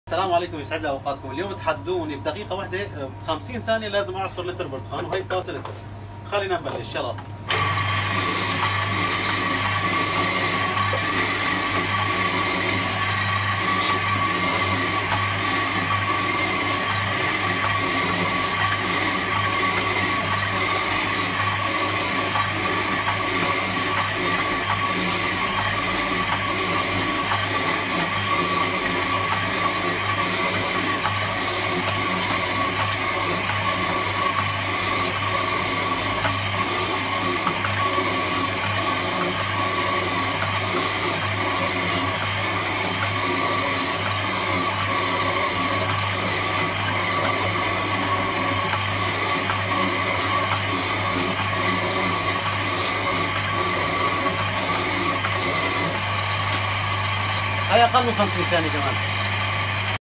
تحدي نار عصر لتر برتقال في 50 ثانية 👀
عصر لتر برتقال 🍊 في اقل من 50 ثانية
صدق او لاتصدق شوف الفيديو للاخير
ملك العصائر ابوحسين يرحب بكم